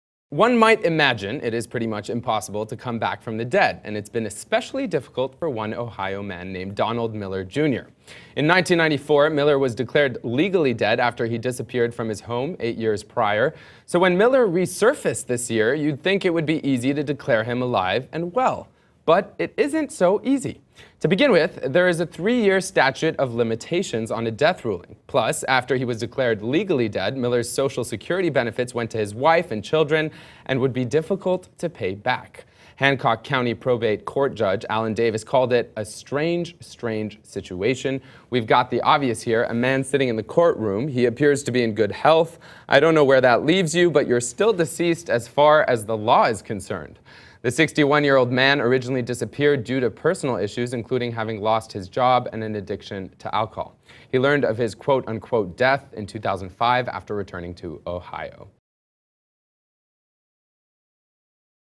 Ohio Judge Tells Man He's Still Legally Dead
FINDLAY, Ohio -- FINDLAY, Ohio (AP) — An Ohio man is finding out there's no easy way to come back from the dead.Donald Miller Jr. went to court this week to ask a county judge to reverse a 1994 ruling that declared him legally dead after he had disappeared from his home eight years earlier. But the judge turned down his request, citing a three-year time limit for changing a death ruling.Hancock County Probate Court Judge Allan Davis called it a "strange, strange situation."
We've got the obvious here. A man sitting in the courtroom, he appears to be in good health," said Davis, who told Miller the three-year limit was clear.
I don't know where that leaves you, but you're still deceased as far as the law is concerned," the judge said.
Miller resurfaced about eight years ago and went to court so that he could get a driver's license and reinstate his Social Security number.
His ex-wife had opposed the move, saying she doesn't have the money to repay the Social Security benefits that were paid out to her and the couple's two children after Miller was declared dead.
Robin Miller said her former husband vanished because he owed big child support payments and that the overdue payments had totaled $26,000 by 1994, The (Findlay) Courier reported.
Miller, 61, who now lives in the northwest Ohio city of Fostoria, told the judge that he disappeared in the 1980s because he had lost his job and he was an alcoholic. He lived in Florida and Georgia before returning to Ohio around 2005.
His parents told him about his "death" when he came back to the state, he said.
"It kind of went further than I ever expected it to," Miller said. "I just kind of took off, ended up in different places."